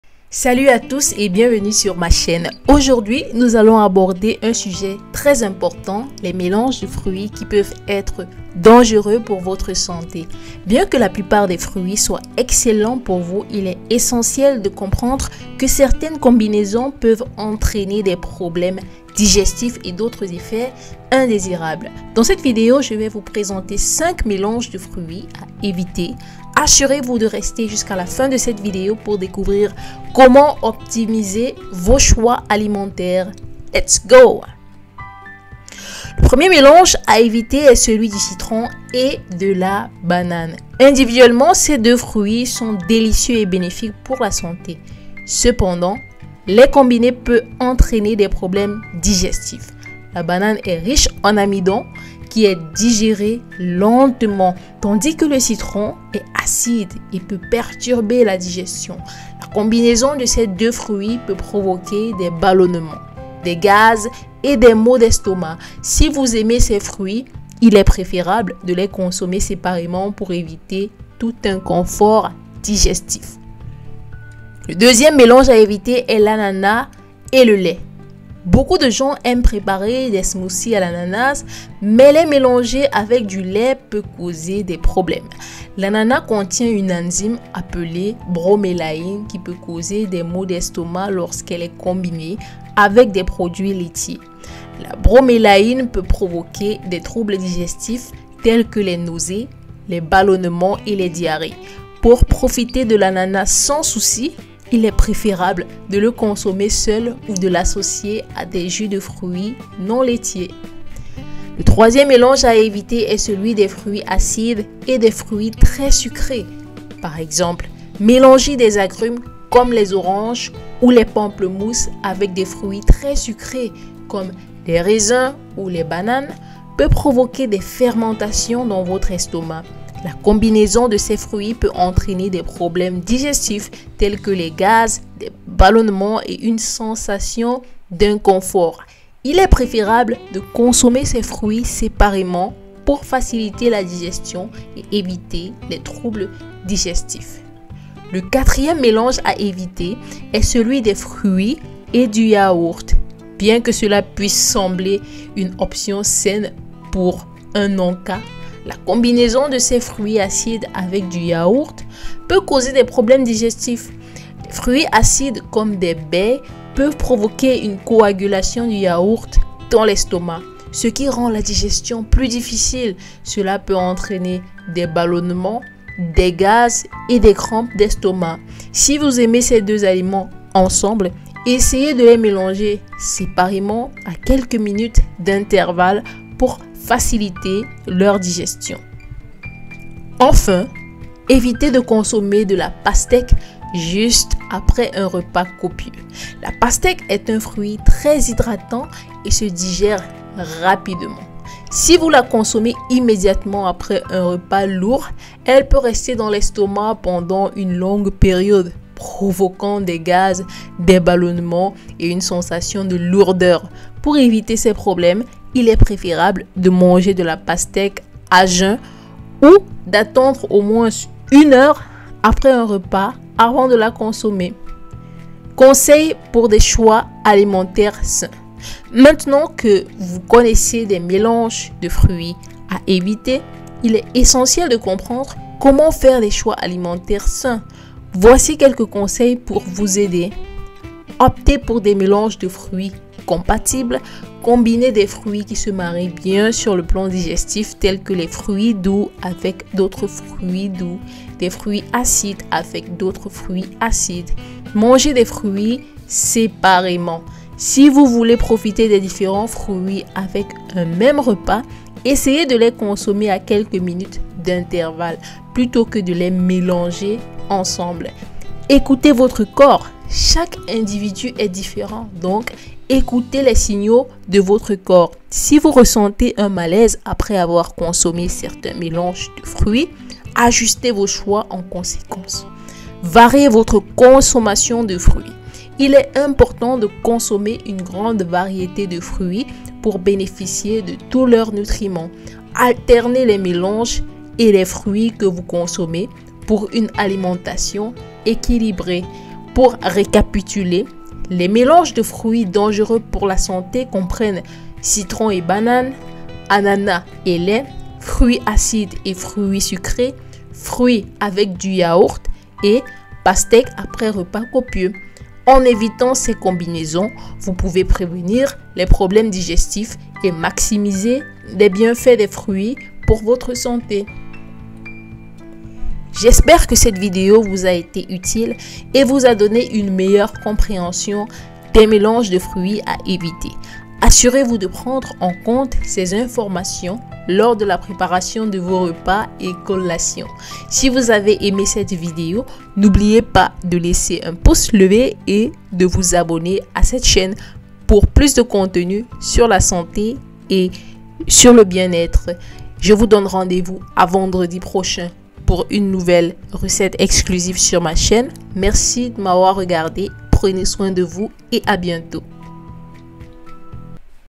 LES MÉLANGES DE FRUITS A NE SURTOUT PAS ESSAYER !!
Dans cette vidéo je te montre les mélanges de fruits dangereux pour ta santé. Bon visionnage.

Matériel de shooting:  samsung S22, Ring light
Logiciel de montage vidéo: Filmora wondershare
Logiciel pour éditer le thumbnail: Photopea